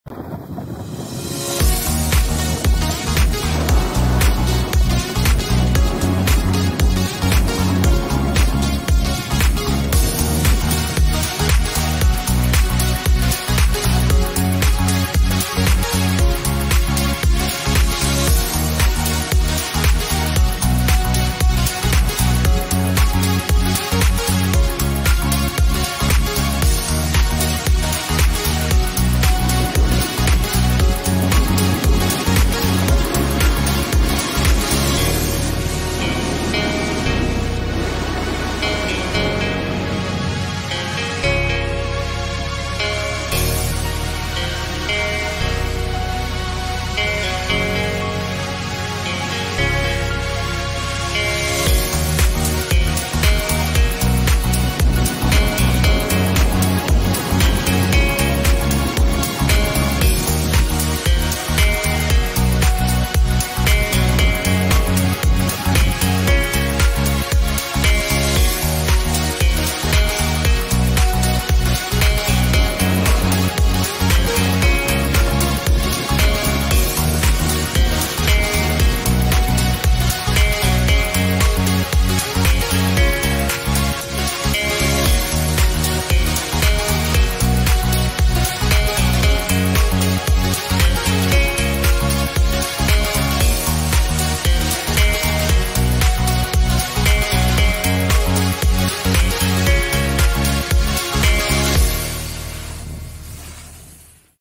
Snow en Sierra Nevada, bajada la loma de dilar
Snow en Sierra Nevada bajada por la loma de dilar con la insta 360